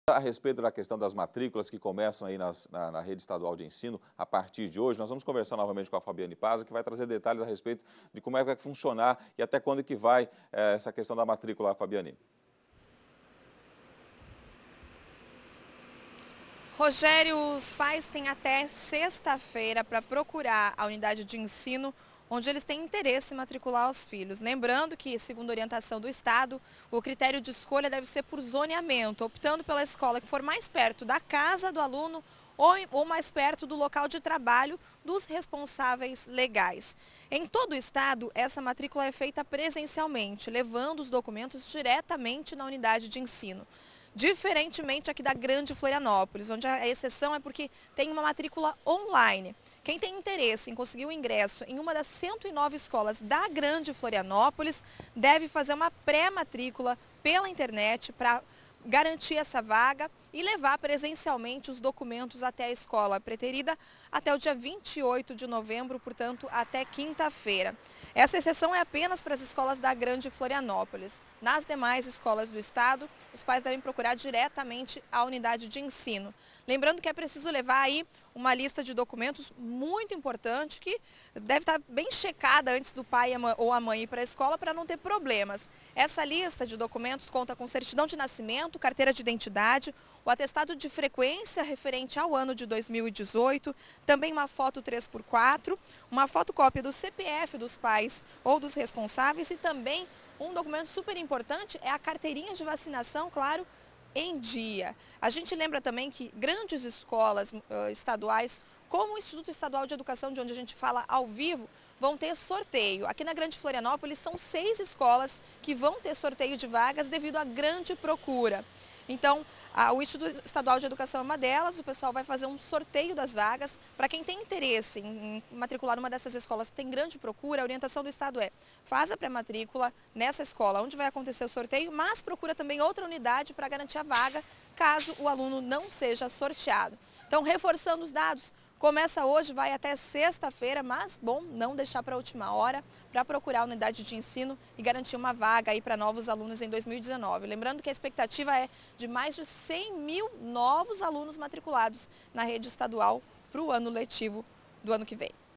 Período de matrículas da rede estadual de ensino inicia nesta segunda-feira
Nesta segunda-feira (19) inicia o período de matrículas da rede estadual de ensino para o ano letivo 2019, em Florianópolis. O prazo irá até 23 de novembro. A escola deve ser escolhida pelos critérios de zoneamento, sendo a mais próxima da residência do aluno ou do trabalho dos responsáveis legais.